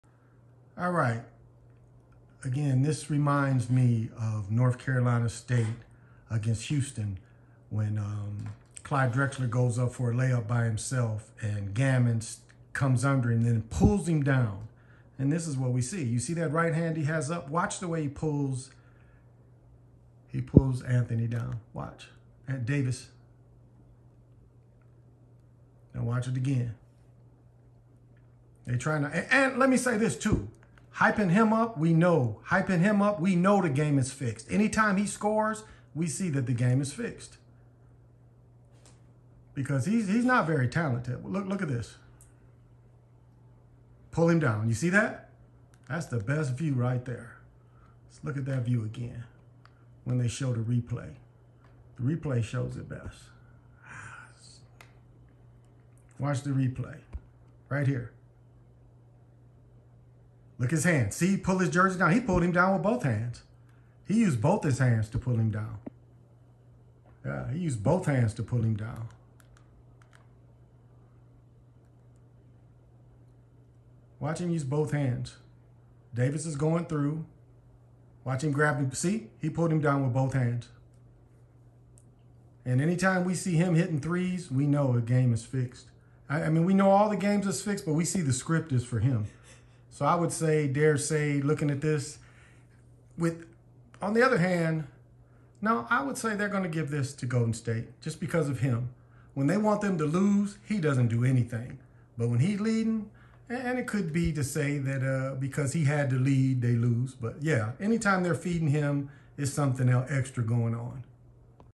Basket Manipulation/ Greens staged game
With Green scoring early we see the script is to highlight the least talented Warrior . At least on offense.